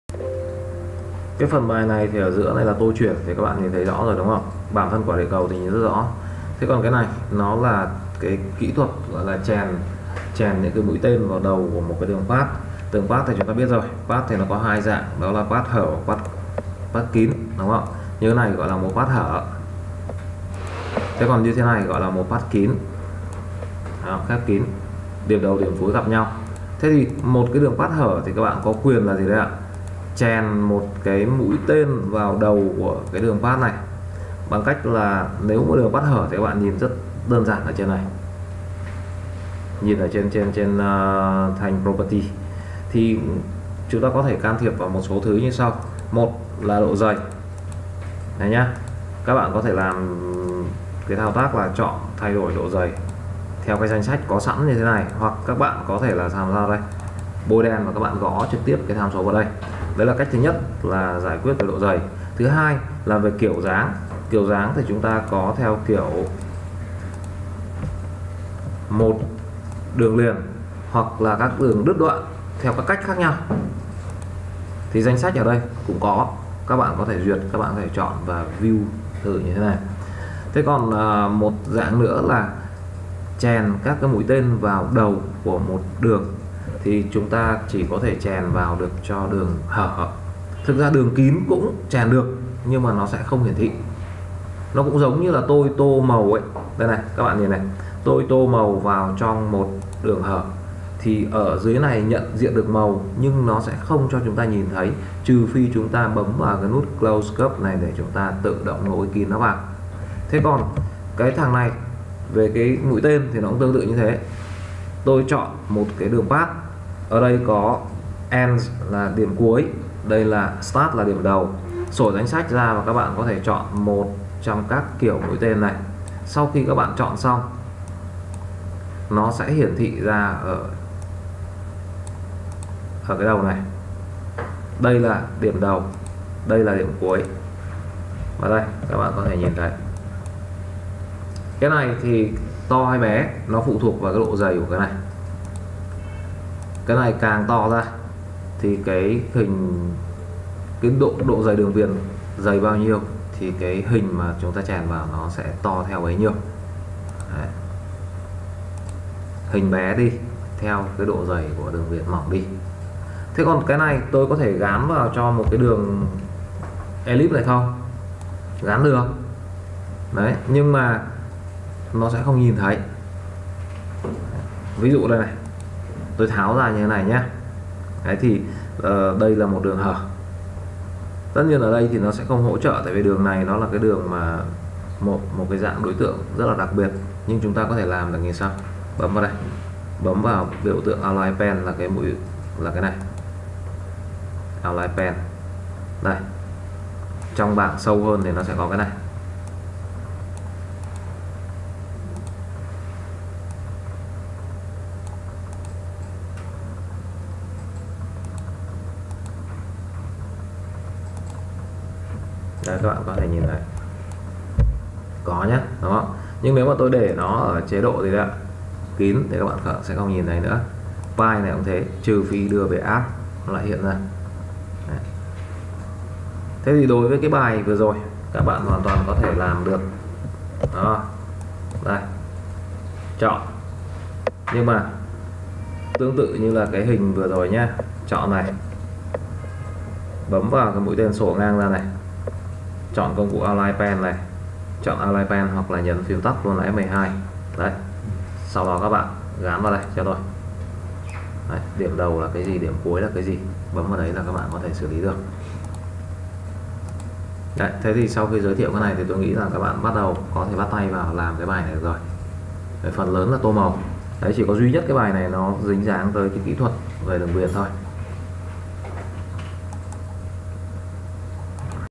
Giáo trình Corel DRAW cho người mới bắt đầu: Chèn biểu tượng vào đường path trong CorelDRAW
Đôi khi các bạn nhìn thấy một số hình ở đầu các đường path trong CorelDRAW. Các biểu tượng đó ở đâu ra, làm cách nào để thêm chúng vào đầu một đường path? Tất cả sẽ được giải đáp sau khi bạn xem video này...
Học CorelDRAW tại Hà Nội cùng giảng viên Phạm Minh Giang